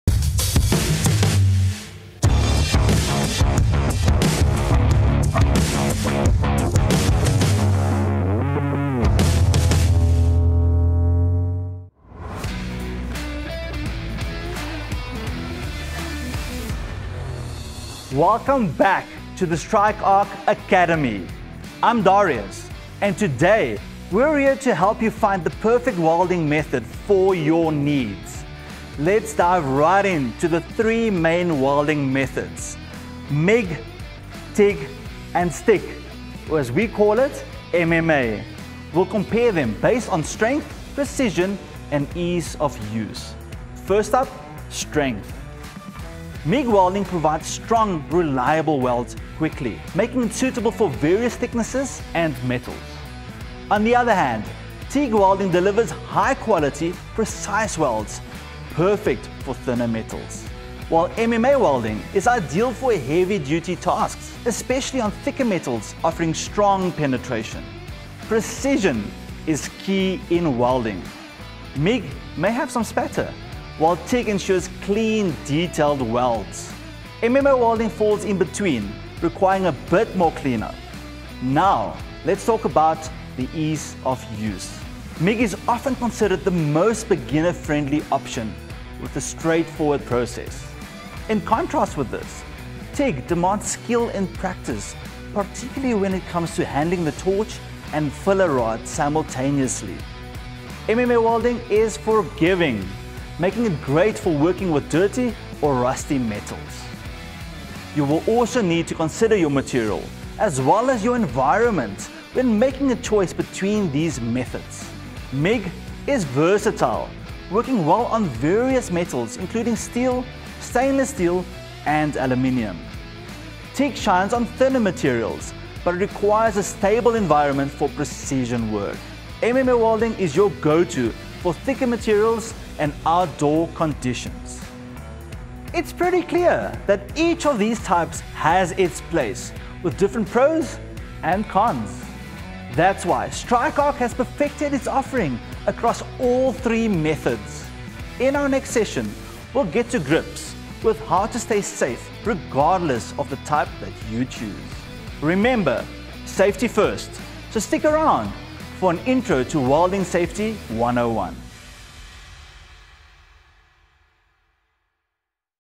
Strike Arc Academy Episode 02 - Choosing the Right Welding Method- MIG, TIG or Stick (MMA)
In this episode, we're diving into the world of welding to help you find the perfect method for your needs. We'll compare the three main welding techniques: MIG, TIG, and MMA (Stick) based on their strength, precision, and ease of use.

Discover which welding method suits your project best:

MIG Welding: Strong and reliable, ideal for various thicknesses and metals.
TIG Welding: Delivers high-quality, precise welds, perfect for thinner metals.
MMA Welding: Perfect for heavy-duty tasks, especially on thicker metals.
Learn about the key aspects of each method:

Strength: Quick, strong welds with MIG, high-quality precision with TIG, and strong penetration with MMA.
Precision: Clean, detailed TIG welds vs. some spatter with MIG and MMA.
Ease of Use: Beginner-friendly MIG, skill-demanding TIG, and forgiving MMA.

Consider your material and environment to choose the best method for your needs.

Join us in our next session for Welding Safety 101. Remember, safety first!